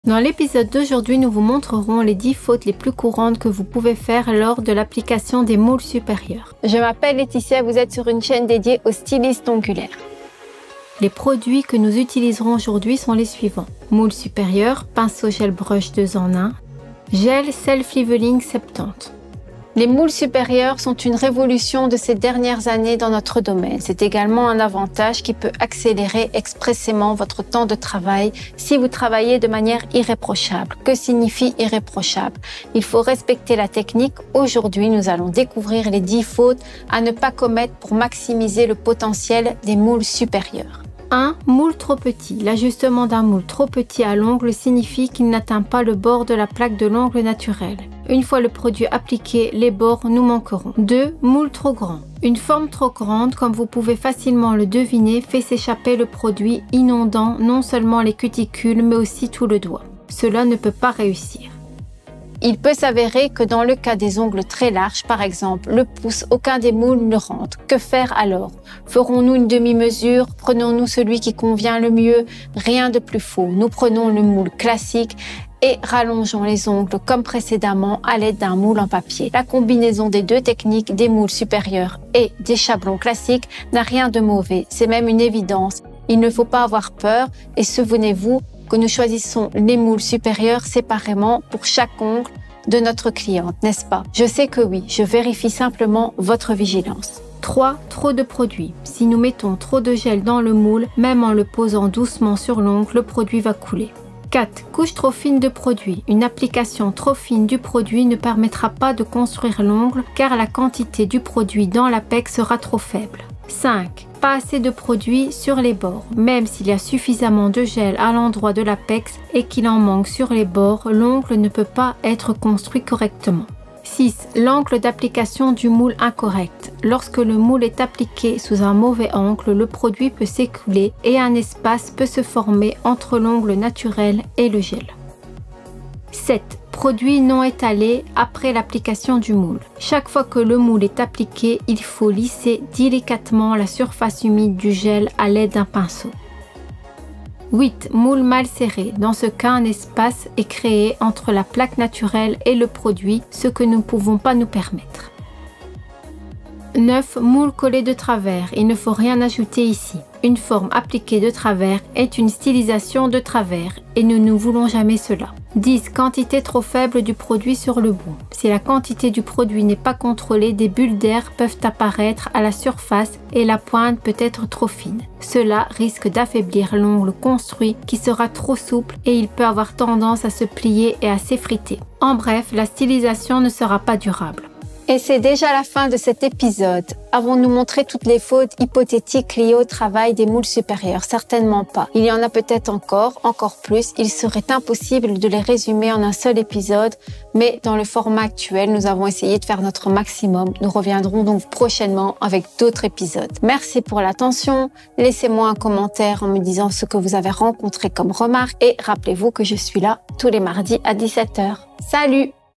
10 ERREURS pendant le travail avec les FORMES SUPÉRIEURES – qu'est-ce qu'il faut éviter?
Dans l'épisode d'aujourd'hui, nous présenterons les erreurs lors du travail avec les formes supérieures ! Qu'est-ce qu'il ne faut pas faire, comment appliquer le gel et travailler avec ? Nous répondrons à ces questions et à d'autres. Vous vous demandez comment faire et surtout comment commencer à rallonger les ongles avec du gel étape par étape, par exemple sur une dual forme ? Marta Olesiak mettra en garde contre les erreurs. Vidéo présentée par Laetitia / Indigo Nails Français.

Le faites-vous déjà ? Génial! Cliquez sur 🔔 pour rester au courant de nos dernières vidéos !
Suivez-nous également sur d'autres chaînes !